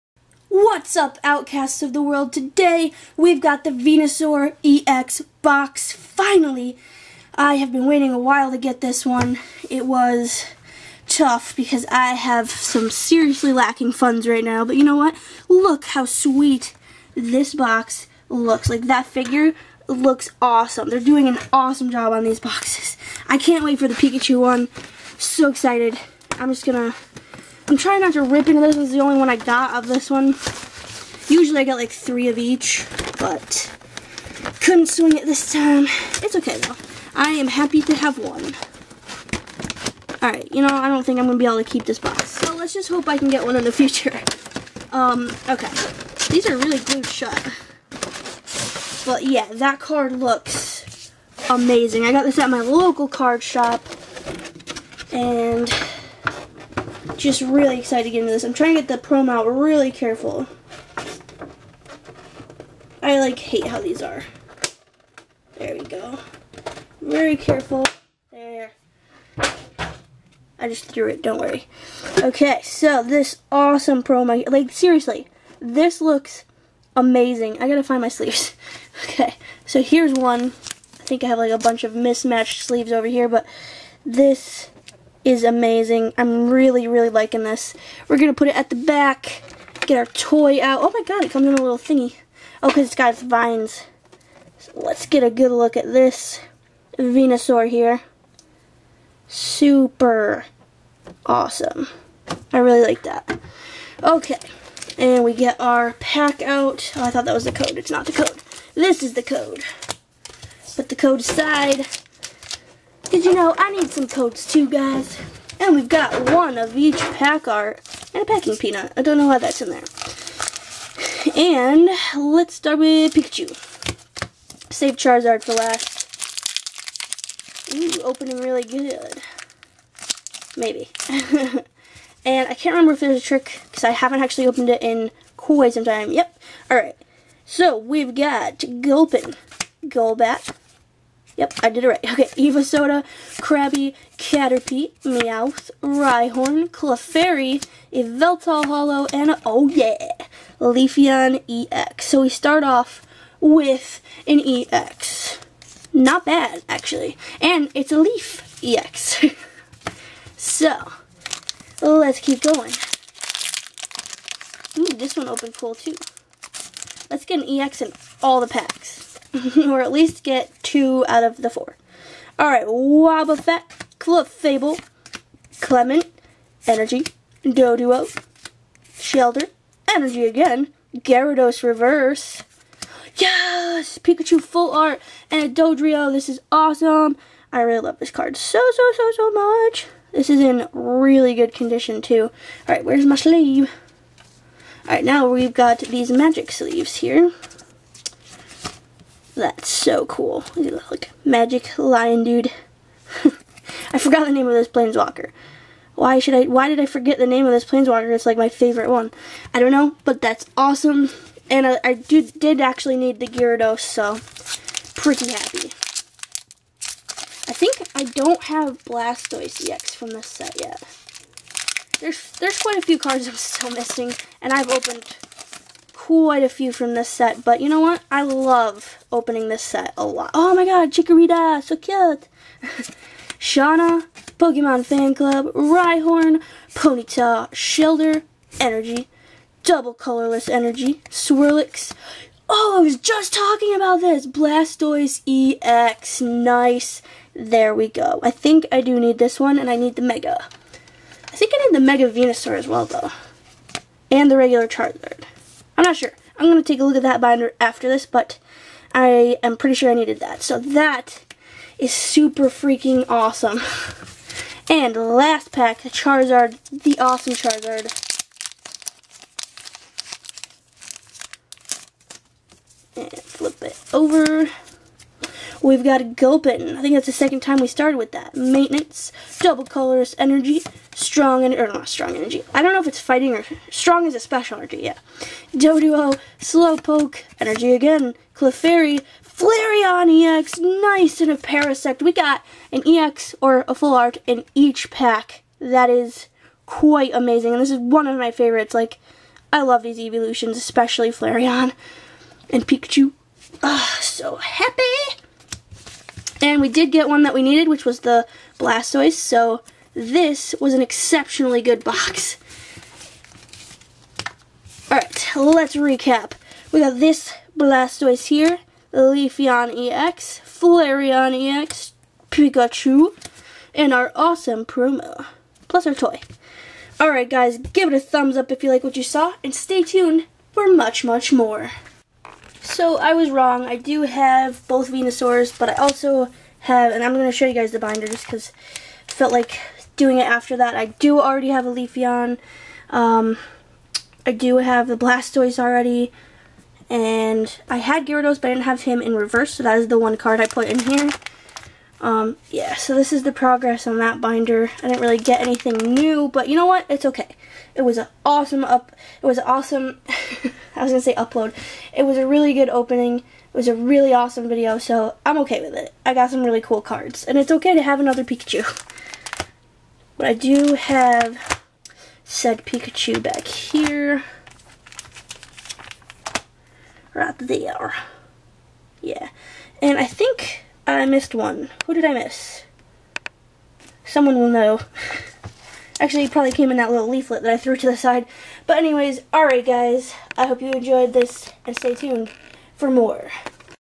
Venusaur EX Mythical Red & Blue Collection Box!!!
Finally opening the venusaur box!! :D